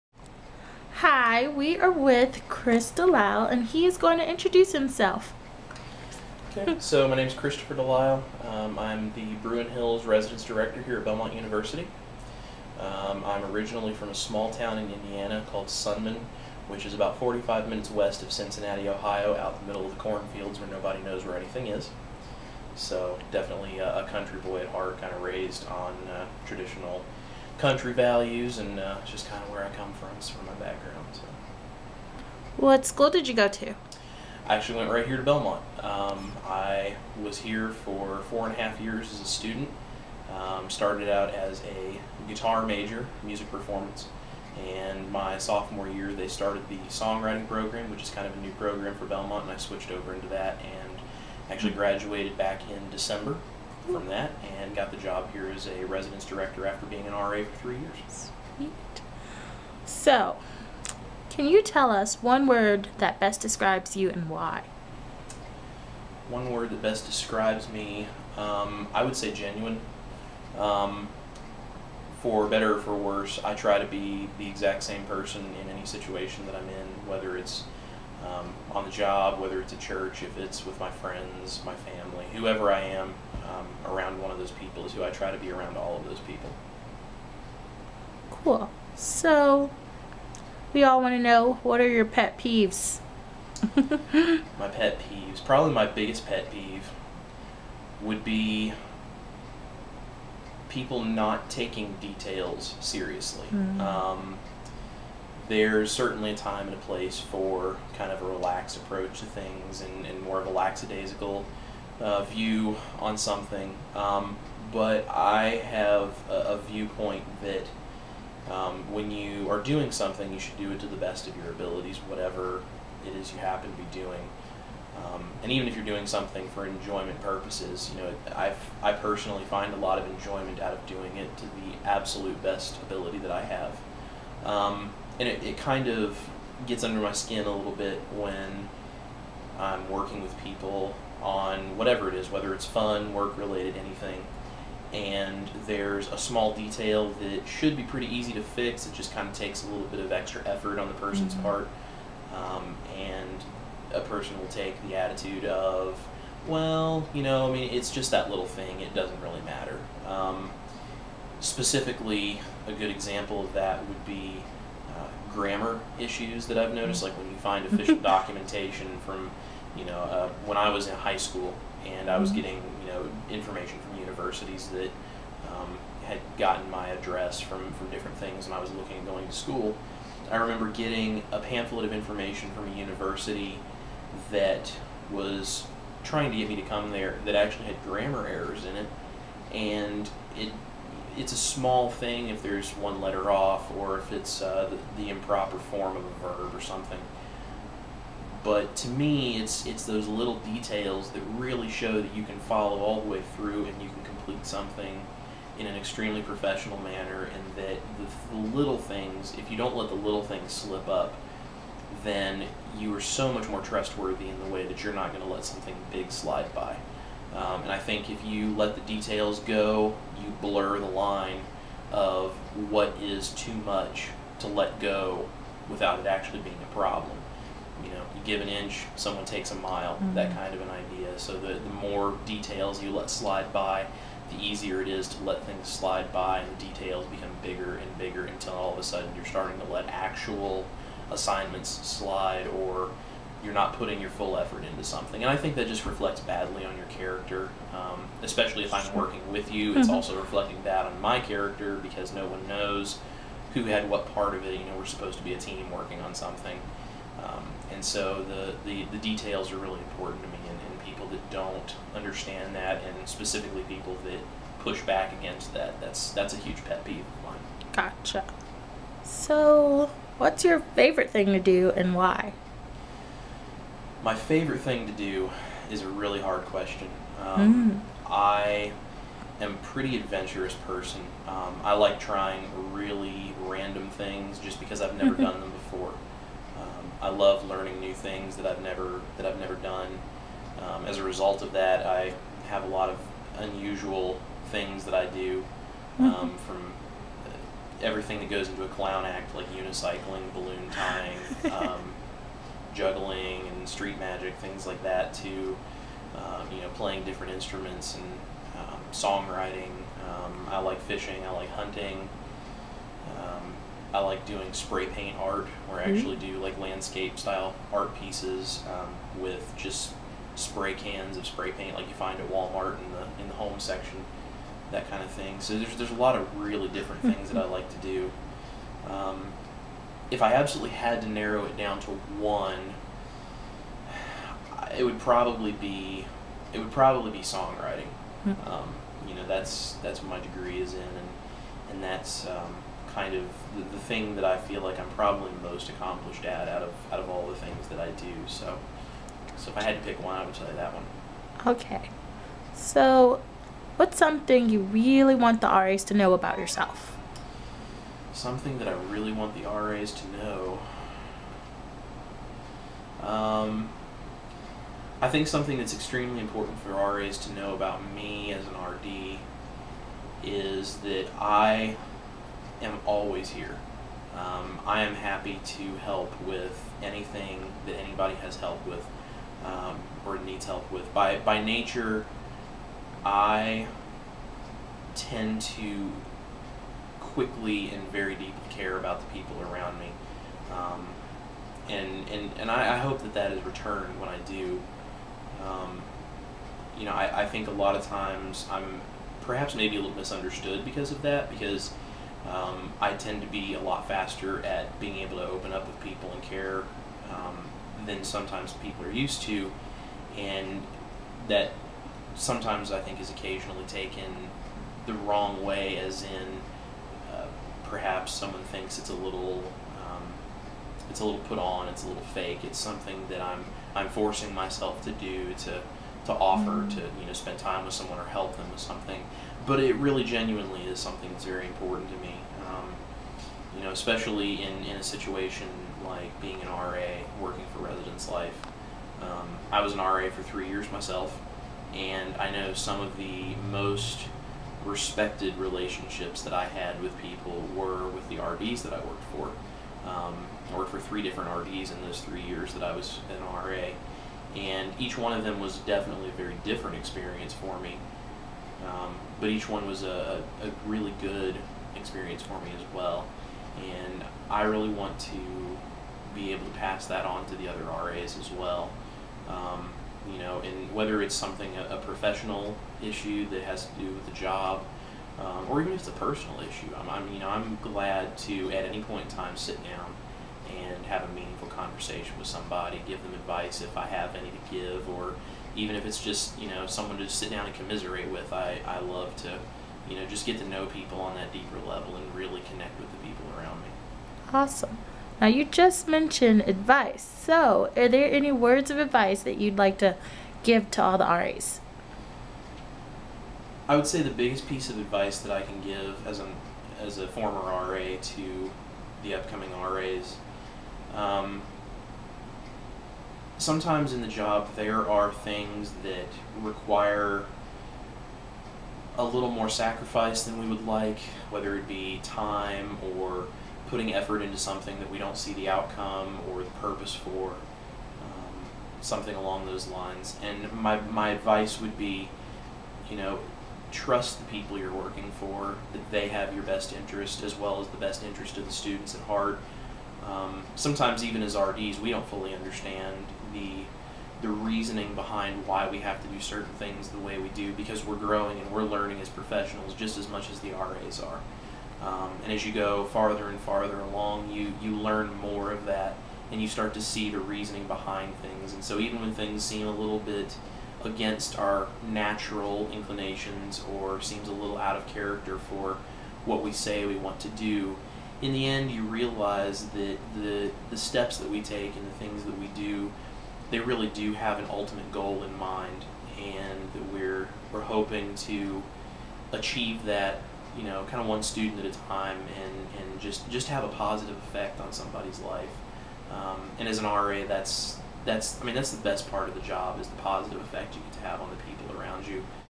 Meet the Belmont RD: Christopher D.
Meet the Bruin Hills Residence Director, Christopher DeLisle, at Belmont University.